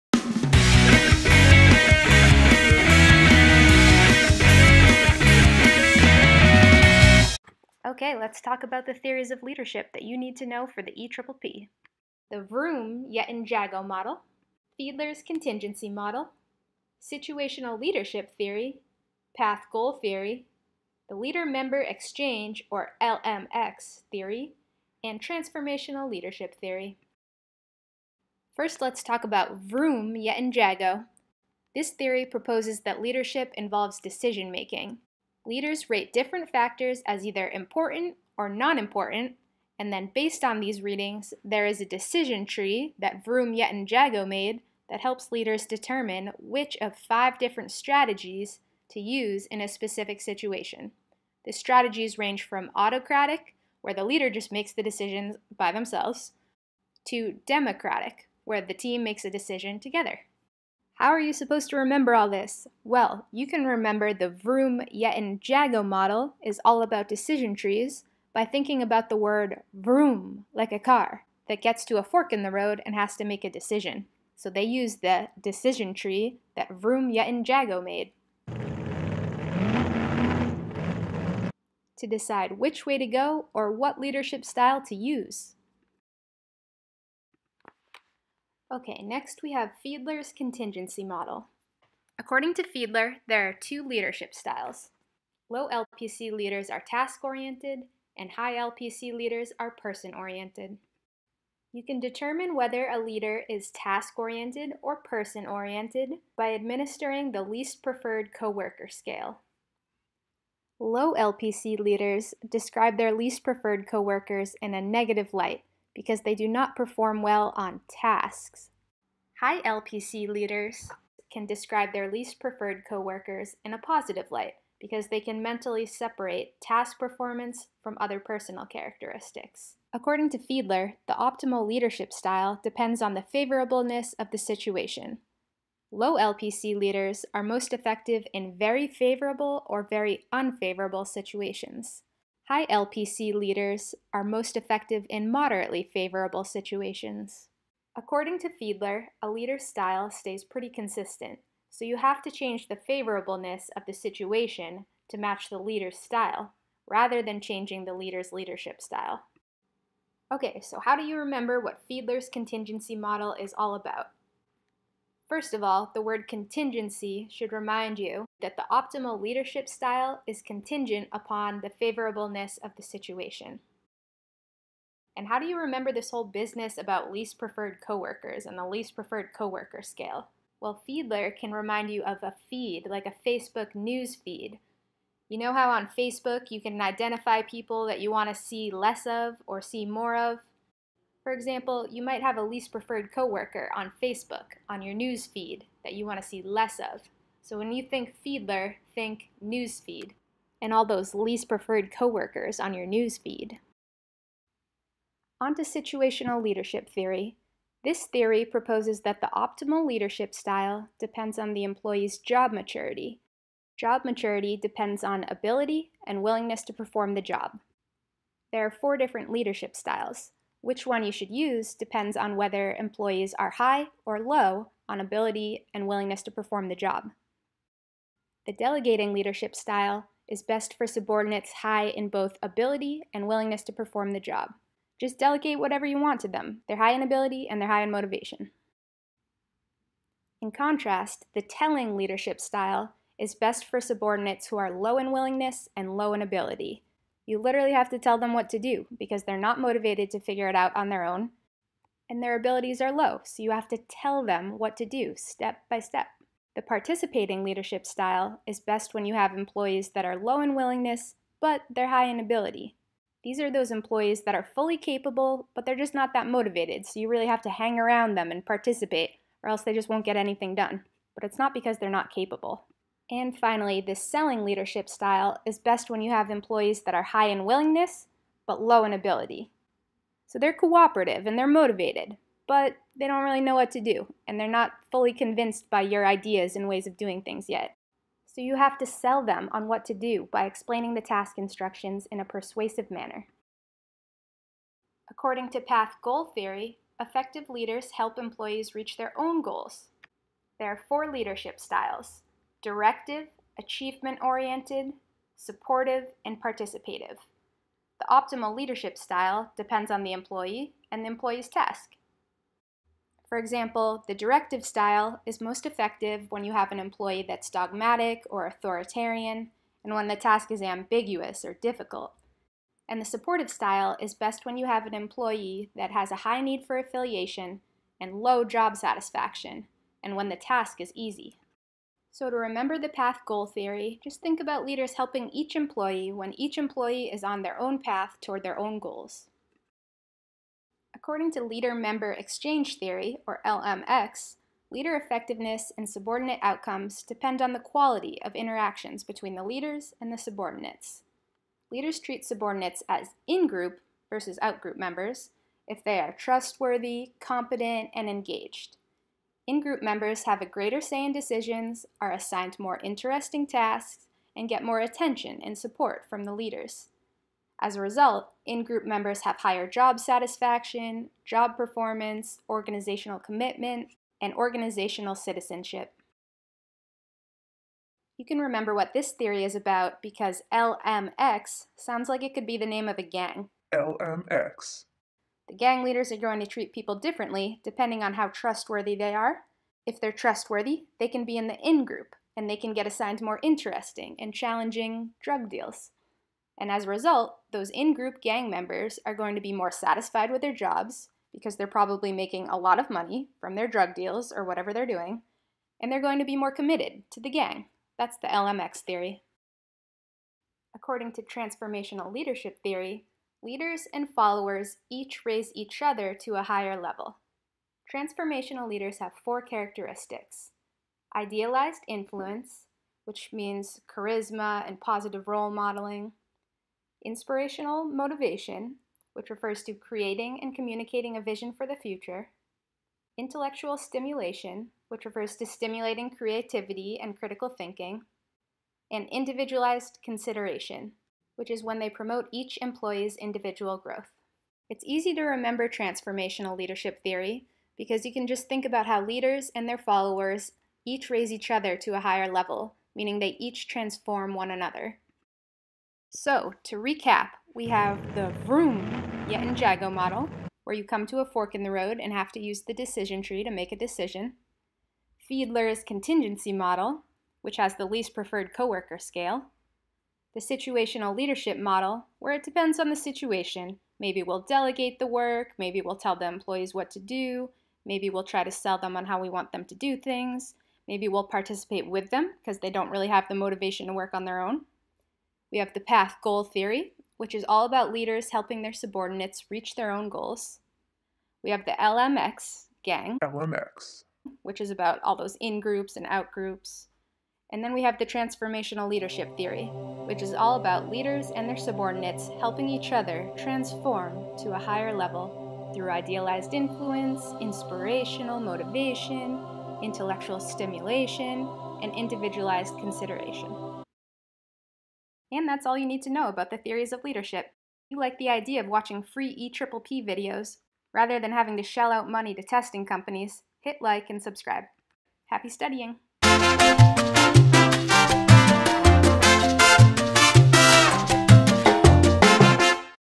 EPPP - Theories of Leadership
-Vroom-Yetton-Jago
-Fiedler's Contingency Model
-Situational Leadership Theory
-Path-Goal Theory
-Leader-Member Exchange (LMX) Theory
-Transformational Leadership Theory